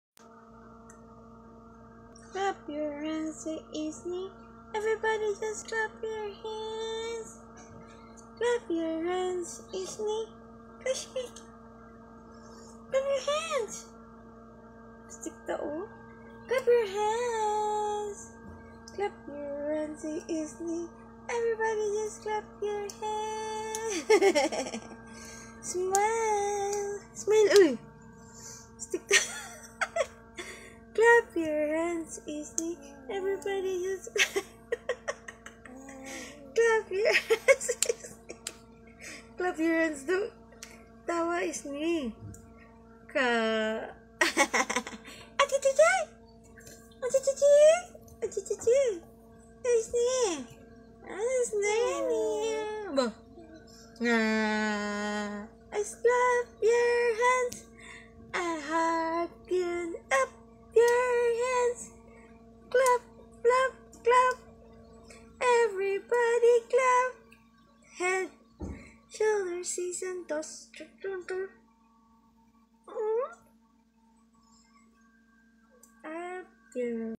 GRUMPY baby | Sobrang TARAY ng batang eto
Disney Dawn is super grumpy. Look at her eyebrows. So strict baby